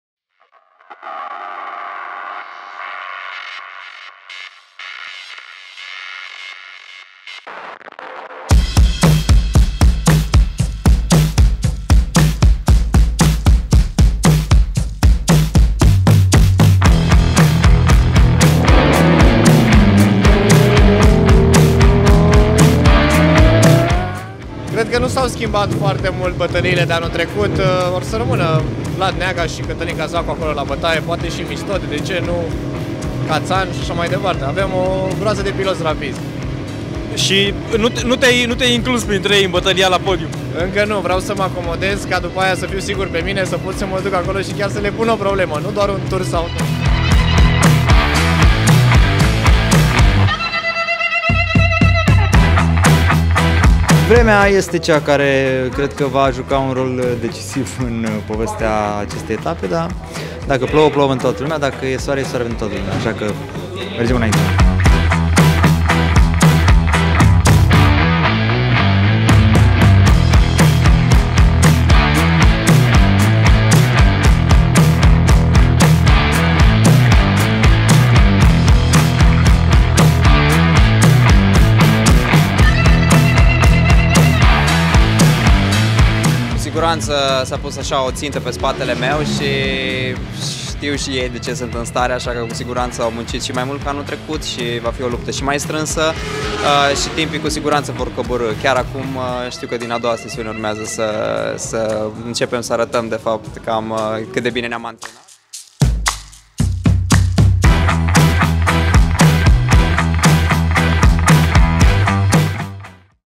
Day 1 - OMV MaxxMotion MotoRC - Etapa 1
De la 🌞 la 🌦, dar o zi de antrenamente libere reușită. Debutul primei etape din OMV MaxxMotion MotoRC a fost pe măsură așteptărilor, așteptări pe care le vom depăși mâine, în ziua calificărilor. Cu certitudine.
Bogdan Buleandra (Team BMR): "Cred că nu s-au schimbat foartr mult bătăliile de anul trecut. O să rămână Vlad Neaga și Cătălin Cazacu acolo la victorie, poate și Ionuț Mistode, de ce nu și George Cațan. Avem o groază de piloți rapizi".

Catalin Cazacu (Racing Academy): "Vremea este cea care cred că va juca un rol decisiv în povestea acestei etape. Dar dacă plouă, plouă pentru toată lumea. Dacă e soare, e soare pentru toată lumea. Așa că mergem înainte".

Vlad Neaga Motorsport (VN Motorsport Racing Team: "Cu siguranță s-a pus o țintă pe spatele meu, știu și ei (n.r. - competitorii) de ce sunt în stare. Cu siguranță au muncit mai și mai mult decât anul trecut și va fi o luptă și mai strânsă", a declarat campionul în exercițiu al clasei Stock 1000.
OMV
BMW Motorrad Romania
VTEX Romania
Redbull Romania